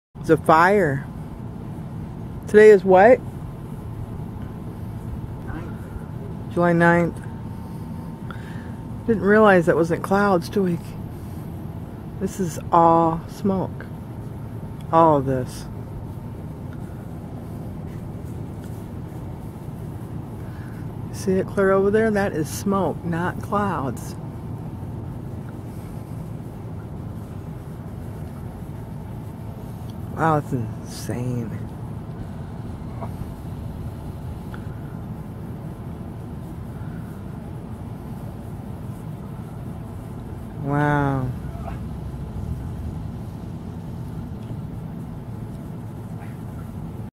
Spokane Long Lake Fire July 9th 2014
Firefighters are battling a fast-moving wildfire in Stevens County in the Long Lake area.

The fire is burning near the intersection of Highway 291 and Highway 231. Guy Gifford from the Washington Department of Natural Resources tells KHQ as of 11:30pm, it is has grown to around 700 acres in size. The fire is burning in a heavily wooded area and Wednesday afternoon, flames could be seen up to 14 feet high above the tree line. There is, as of this upload, 0% containment.  :-(  This video was shot near Argonne and Bigelow Gulch in Spokane at 7:53 PM.